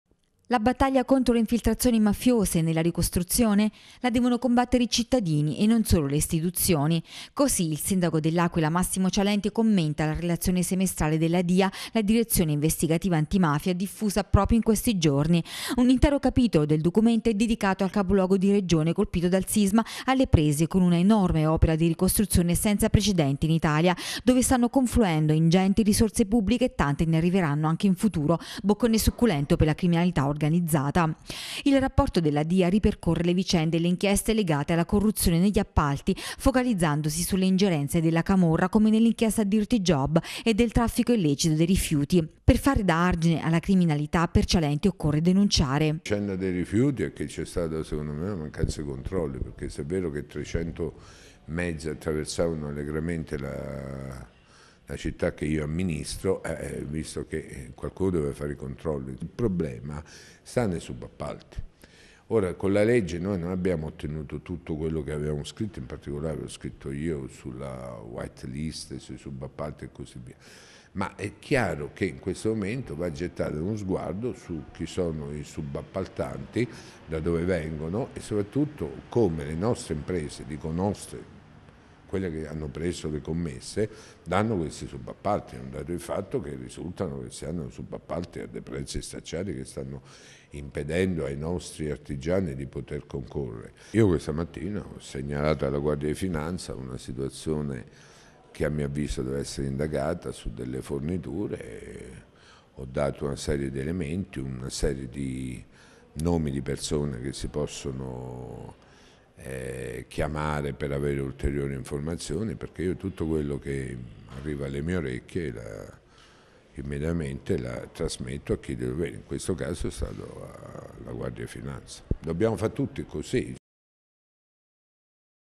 L’Aquila - “Contro la Camorra servono gli occhi dei cittadini”
Il sindaco dell’Aquila commenta il rapporto della Dia: per Cialente il malaffare si combatte con le denunce.